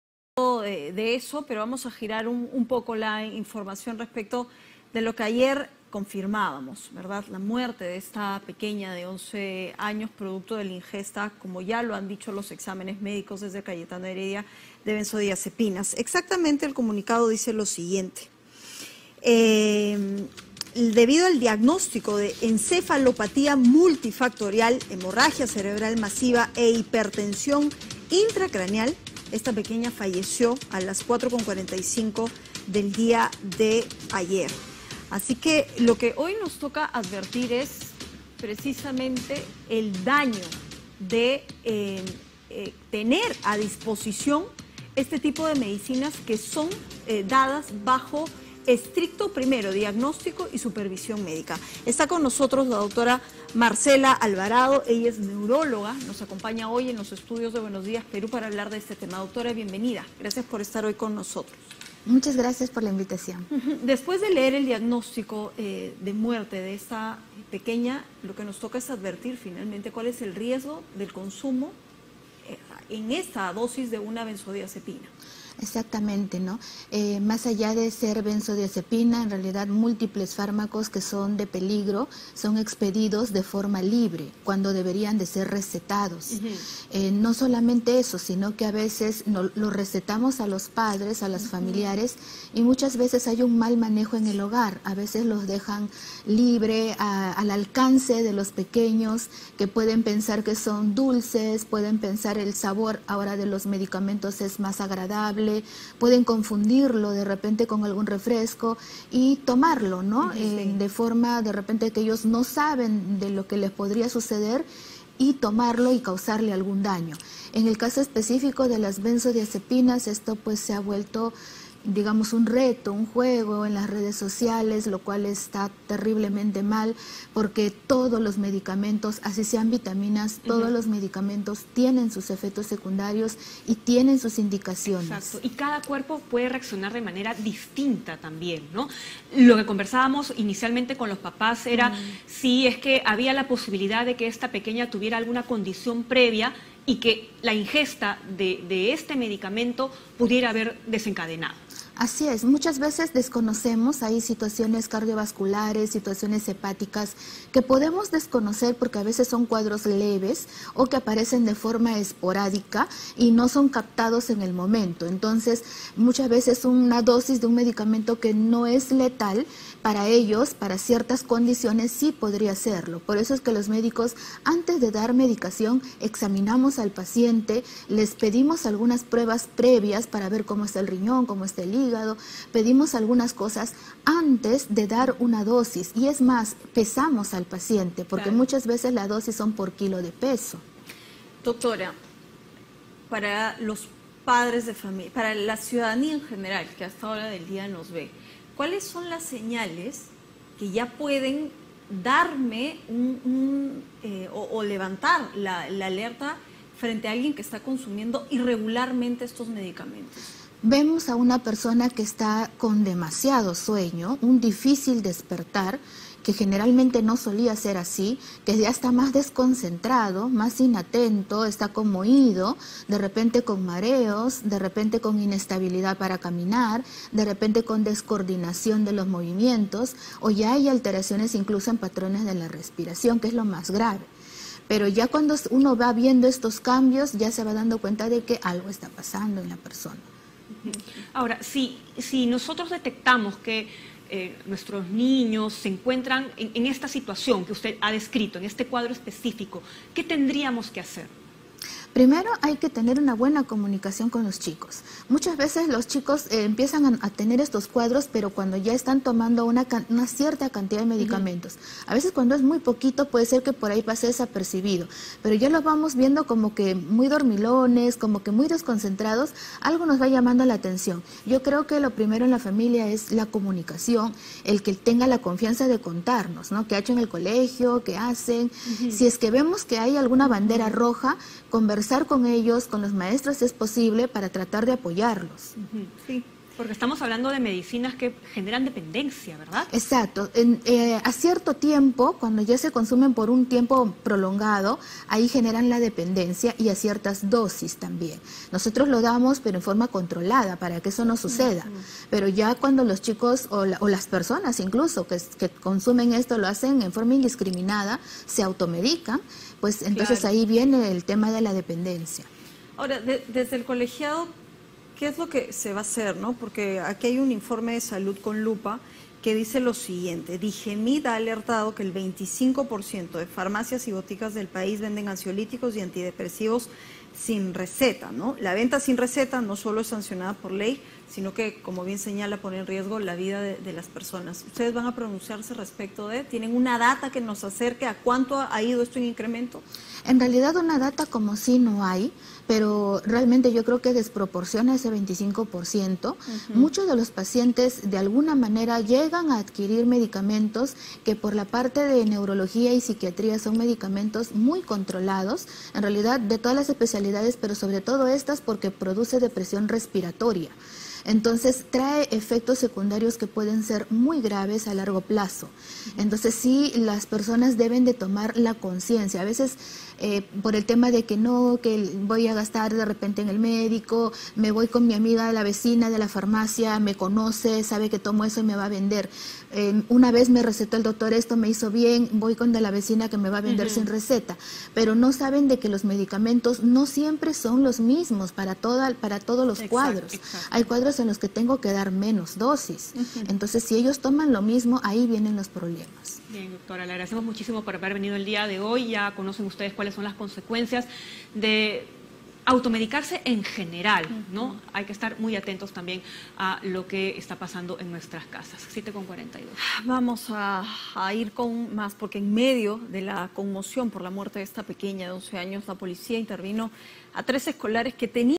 Efectos de la benzodiazepina . Dra. Marcela Alvarado Neuróloga.
¿ Cuáles son los efectos de la benzodiazepina?

Dra.  Marcela Alvarado Neuróloga.